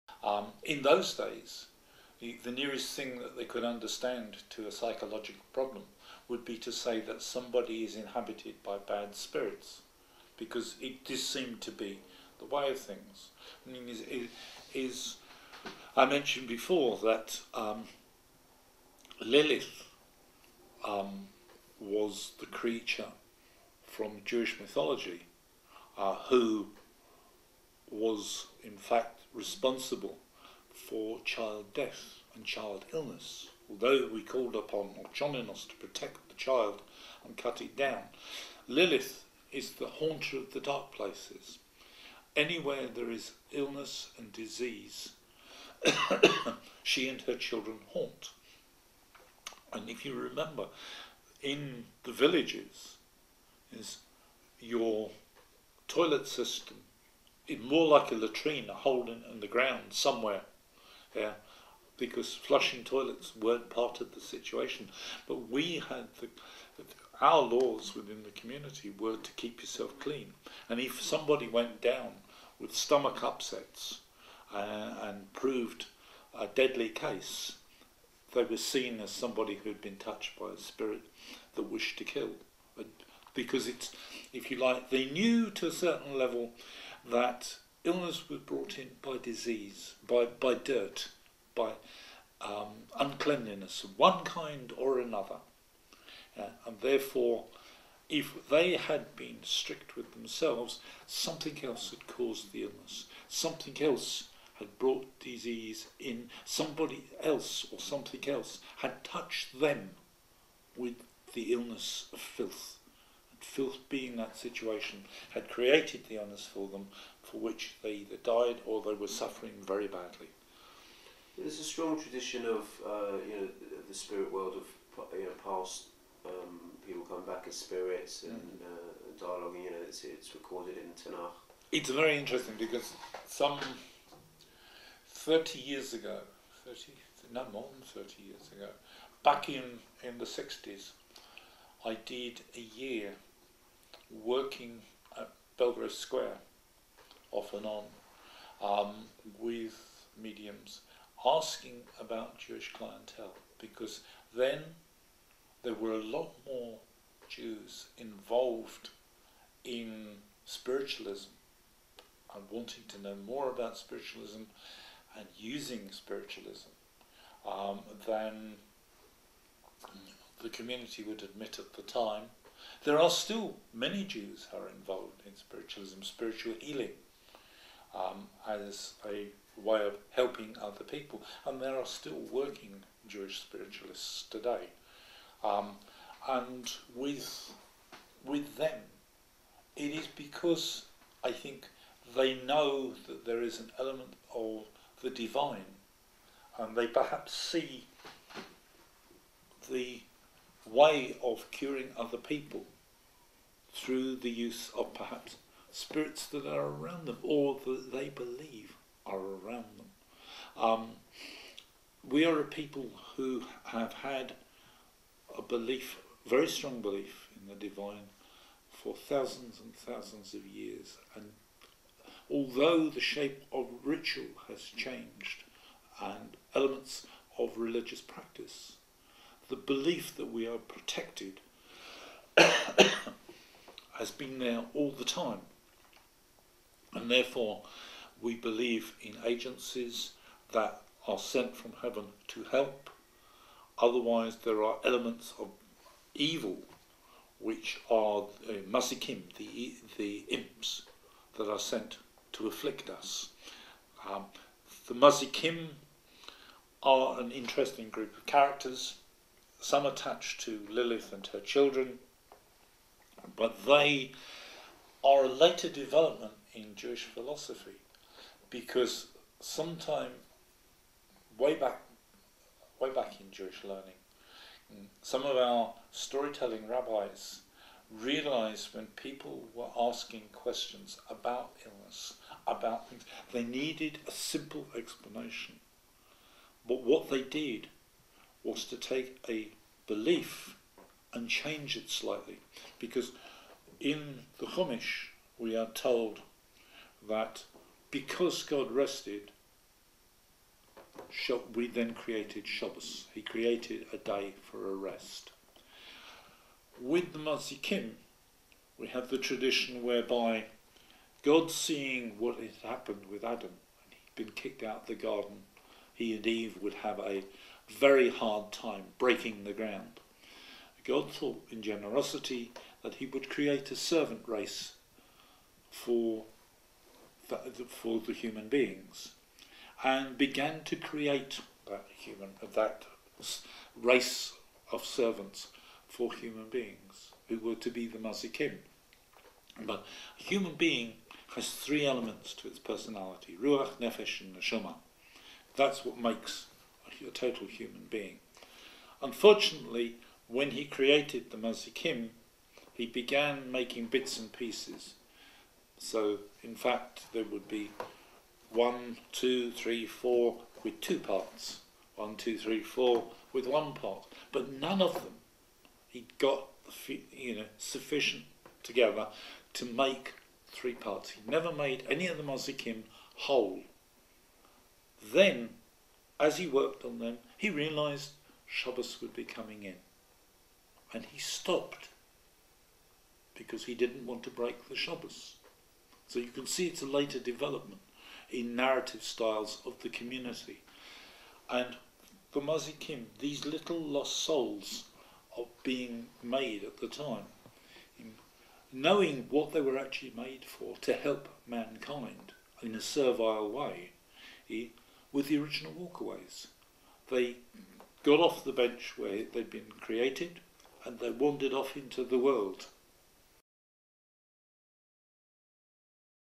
COTG Derek Reid "supernatural explanations of psychology"
Derek Reid is a Jewish poet and folklorist who specializes in Ashkenazi traditions, Yiddish folk music, oral storytelling, and Jewish superstitions.
His core passion concerns preserving the rich cultural heritage of the Jewish East End of London.
Children of the Ghetto is an archival movie project documenting histories of the Jewish East End of London.